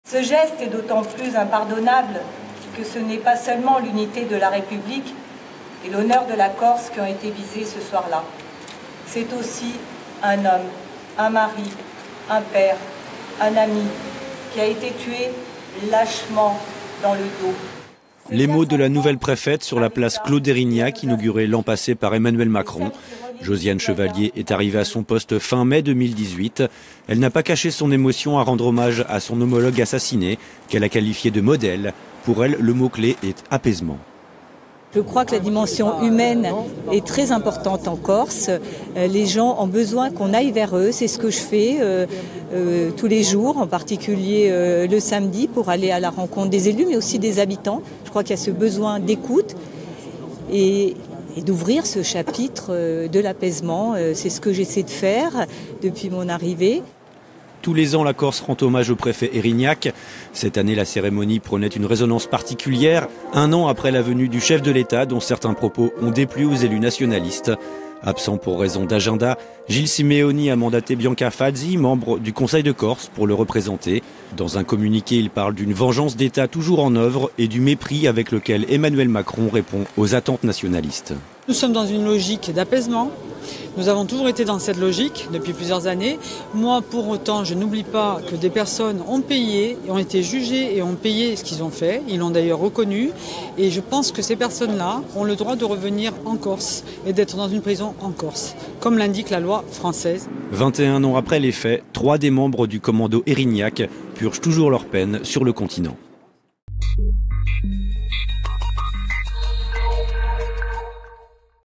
Ajaccio : cérémonie d’hommage au préfet Erignac, 21 ans après son assassinat
Ce mercredi 6 février se tient une cérémonie d’hommage au préfet Claude Erignac à Ajaccio. Il y a été assassiné en 1998.

Intervenants - Josiane Chevalier, Préfète de Corse ; Bianca Fazi, Conseillère exécutive.
Equipe - Damien Lefauconnier ; Franck Rombaldi ; Mattea Luccioni.